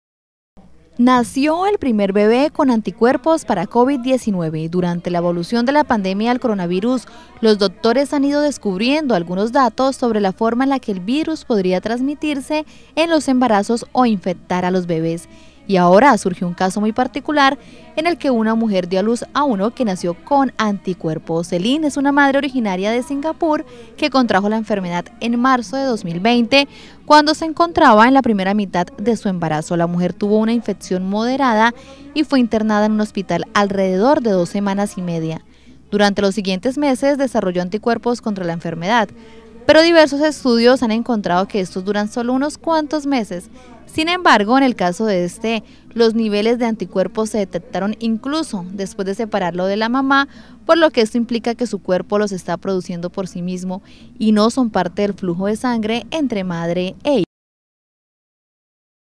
Nació bebé inmune al Covid-19
Internacional ➟ La pandemia de Covid19 es materia de estudio para la ciencia, pues recientemente se conoció un caso de inmunidad en un bebé cuando la progenitora padeció el virus en estado de gestación.